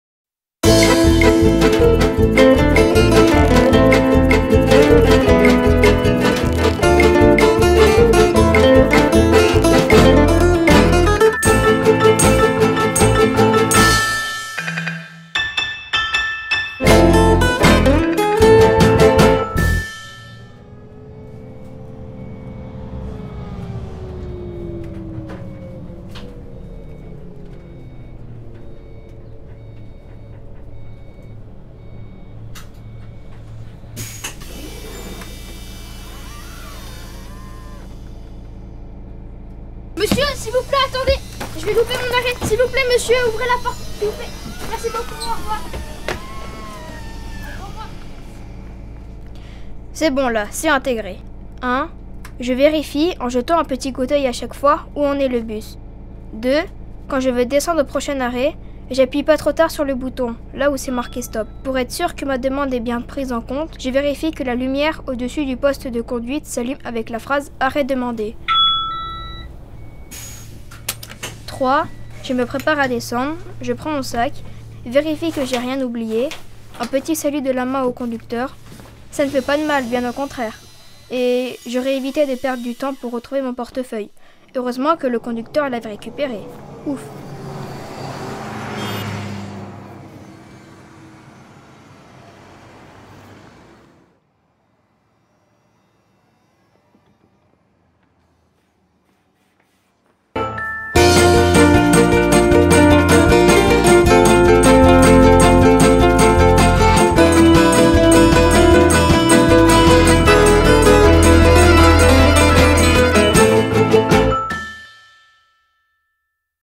Voyager en sécurité, c'est pas compliqué - Épisode 9
Découvrez le neuvième épisode de cette websérie où des jeunes collégiens du Grésivaudan jouent leur propre rôle d'usager des transports en commun.
Chaque épisode porte un message clé. Le neuvième s'intitule :évite les mauvaises surprises, anticipe ta descente.